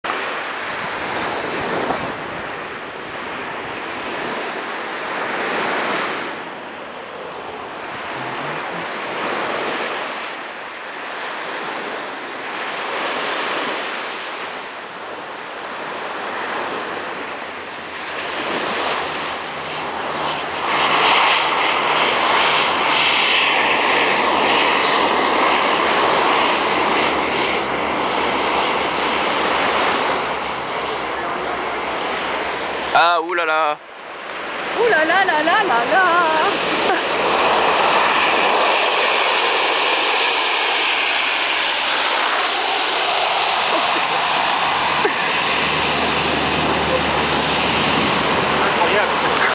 Canadair low passing and sealanding. Bastia, Corsica
The second landing is much more impressive